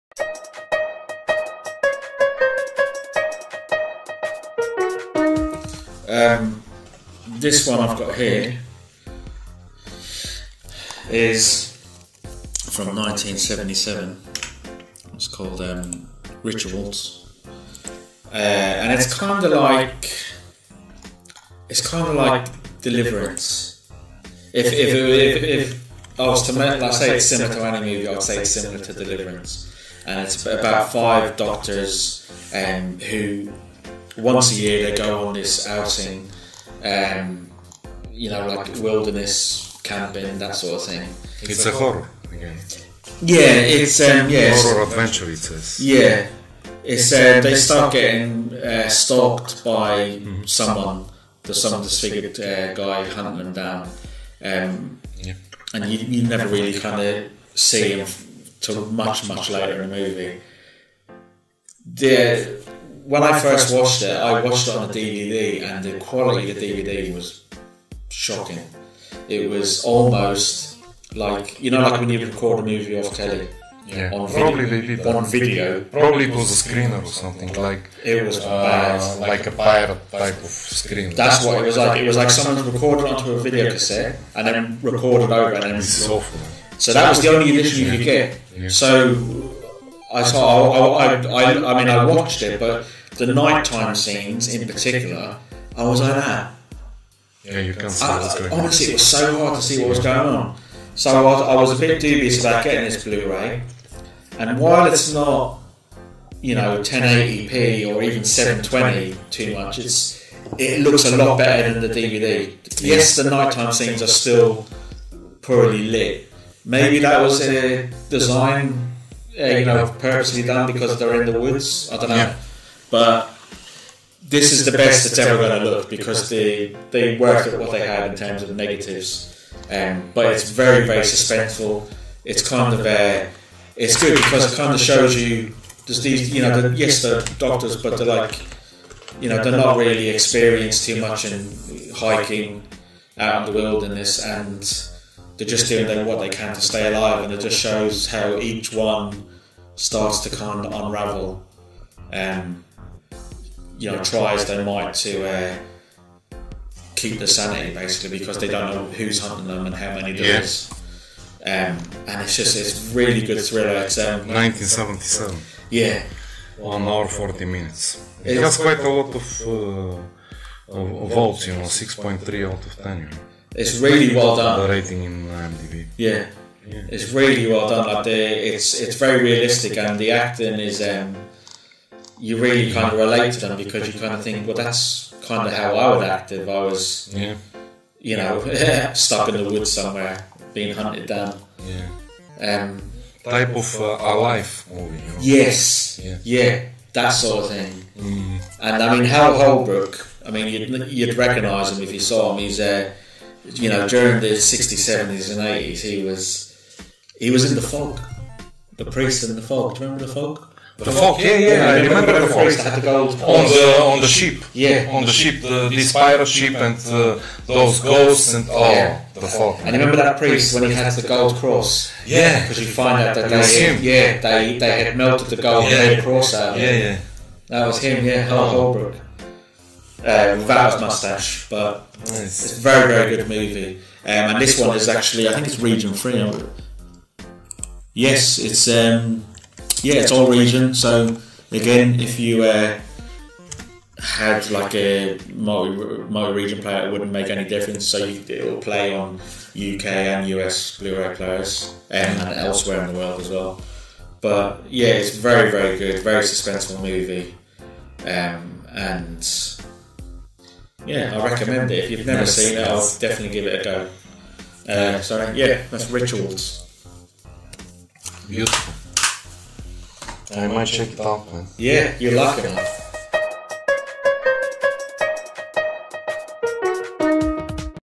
Rituals Review – 1977 movie (part of Episode 17) Director Peter Carter Adventure Horror Thriller
Rituals was release in 1977. It is an Adventure Horror Thriller movie by the director Peter Carter.